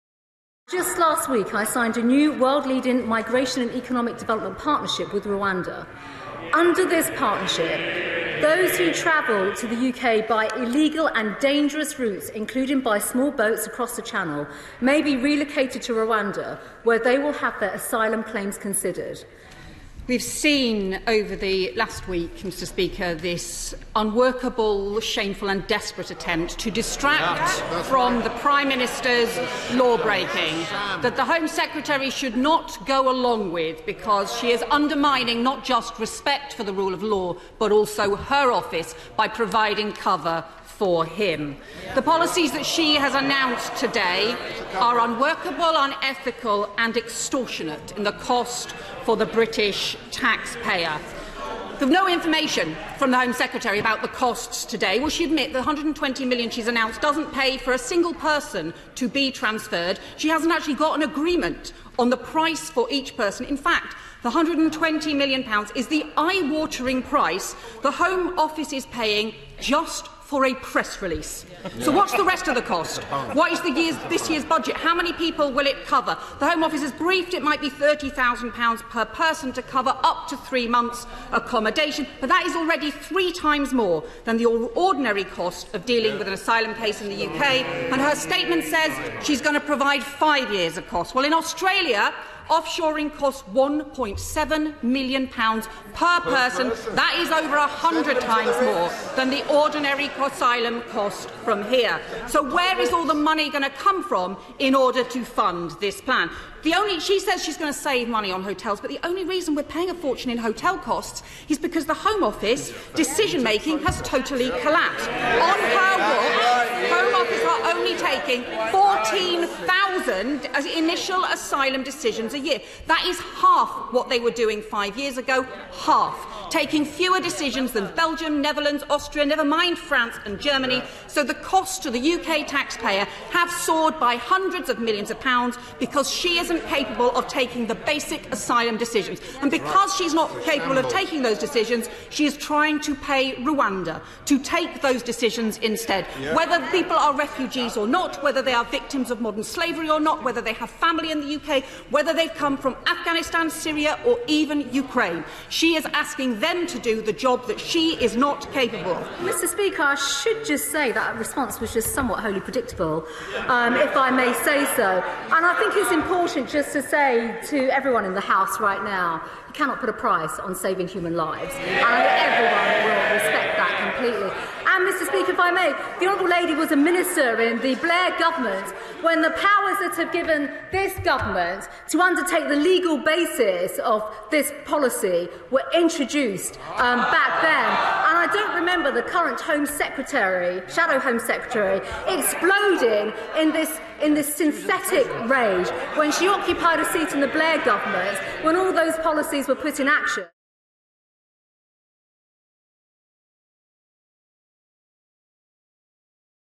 'Unethical and extortionate': Yvette Cooper challenges Priti Patel on Rwanda asylum plan
The home secretary, Priti Patel, was on Tuesday challenged by Labour's Yvette Cooper on the government's new scheme to send people seeking asylum in the UK to Rwanda.

The shadow home secretary denounced the policy as unworkable and unethical, as well as costly for taxpayers. Britain and Rwanda announced last week they had struck an agreement that will see some people arriving in the UK as stowaways on trucks or in small boats sent 4,000 miles to the east African country, where their asylum claims will be processed and, if successful, they will stay in Rwanda